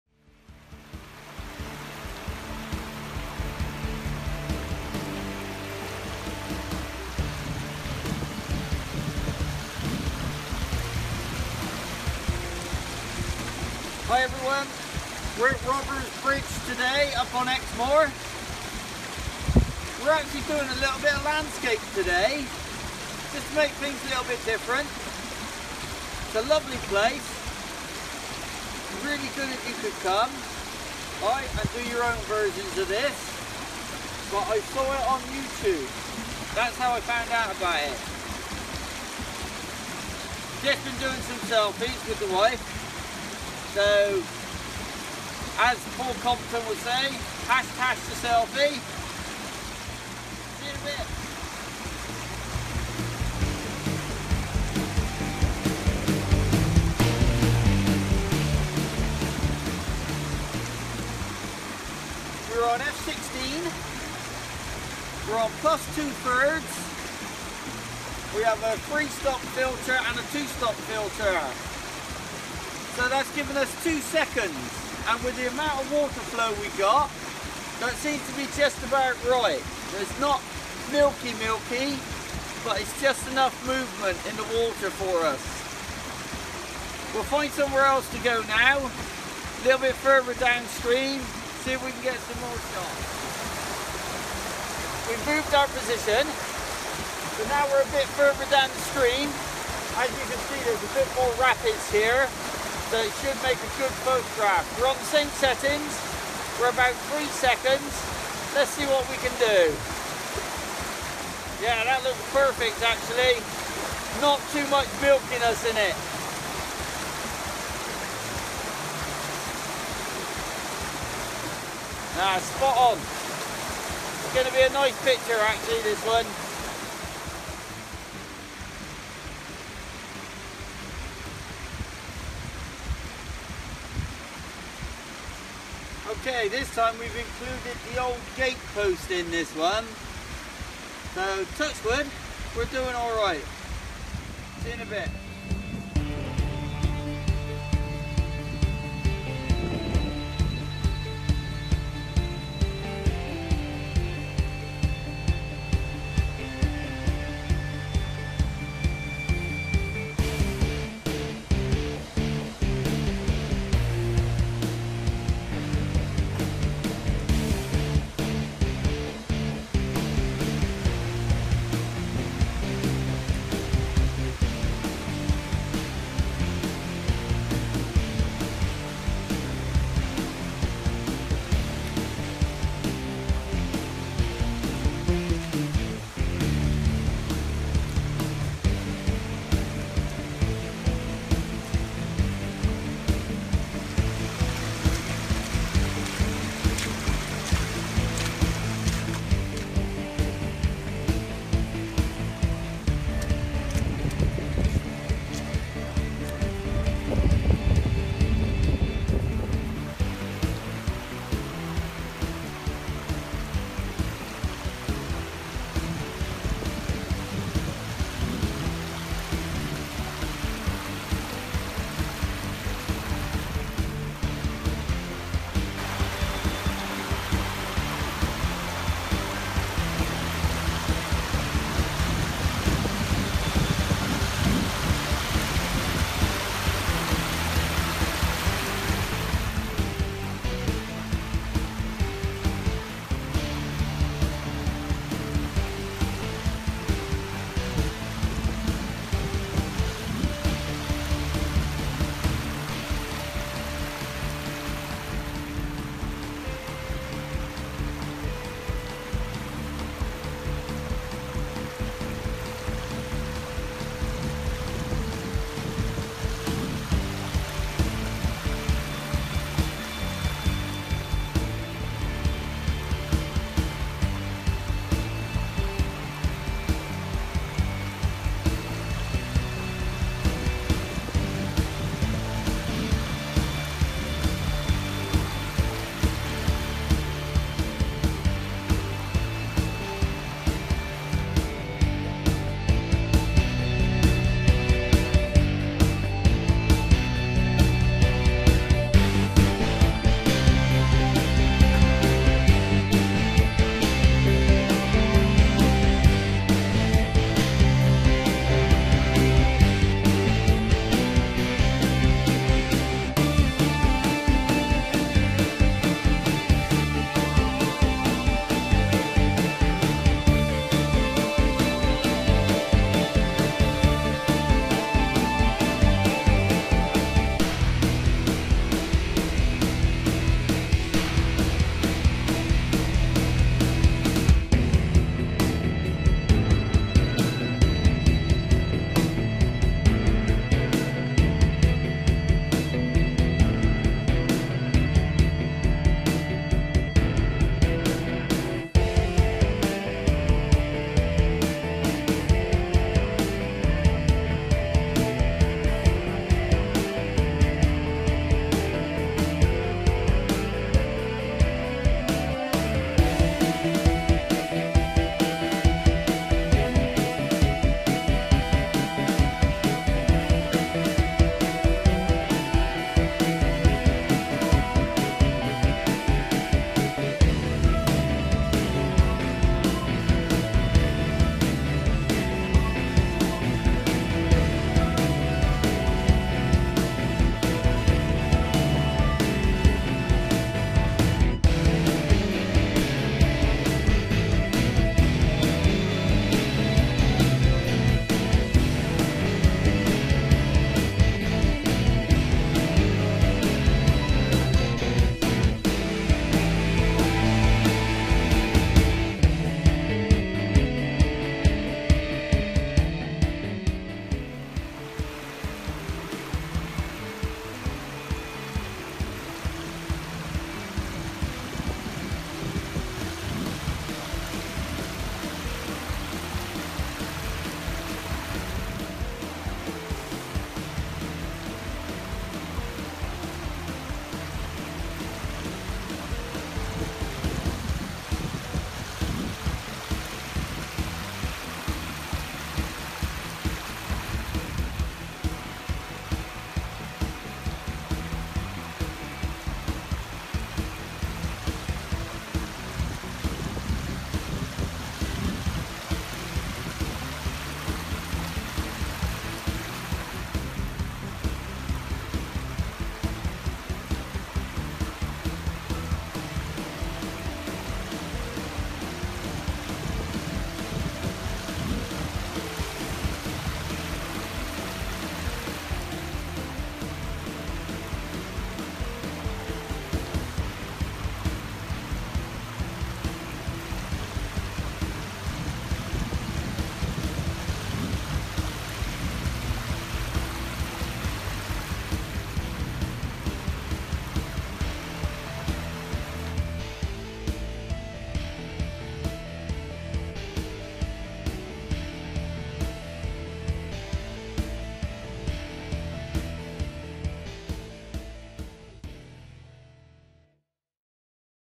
Robbers bridge Exmoor
Went to Robbers Bridge on Exmoor to try something different some landscape photography then on to Horner wood. I will going back to Horner wood soon.